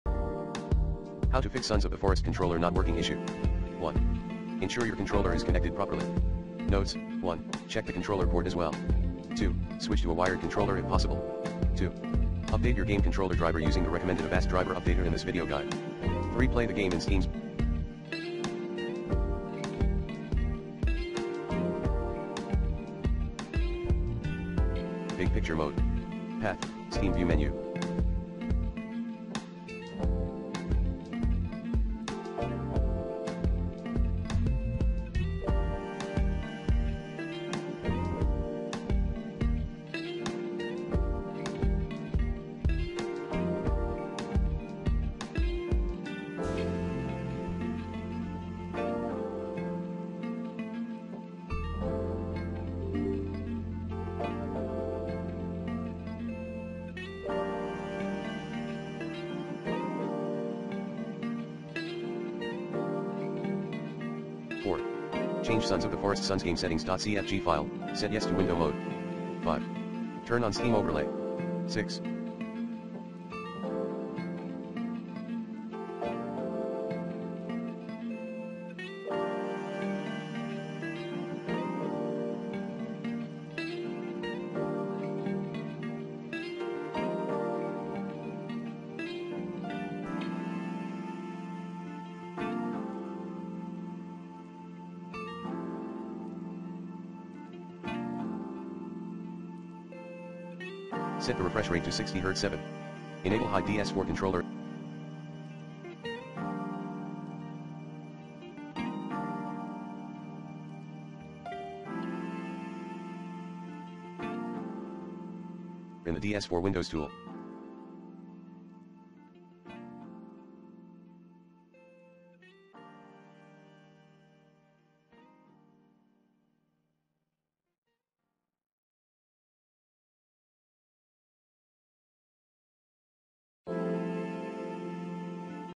Fix Sons of the Forest Controller Not Working
Here's how to Fix Sons of the Forest Controller Not Working. Run Avast Driver Updater@ to keep your device drivers up-to-date, easily & effectively.

If you are experiencing an issue in Sons of the Forest where your controller is not working, here are some things you can try to fix the problem:

1. Check your controller: Make sure your controller is properly connected and turned on. If you are using a wireless controller, try replacing the batteries or charging it.

2. Check game settings: Make sure that your controller is selected as the input device in the game's settings. You can usually find this option in the game's options menu.

3. Restart the game: Sometimes, simply restarting the game can resolve controller issues.

4. Update drivers: Make sure your controller's drivers are up to date. If you are playing on PC, you can usually download the latest drivers from the manufacturer's website.

5. Try using a different controller: If you have another controller available, try using it to see if the problem persists.

6. Try using keyboard and mouse: If all else fails, try playing the game with a keyboard and mouse to see if it resolves the issue.

If the issue persists even after trying these solutions, you may want to reach out to the game's developer or support team for further assistance.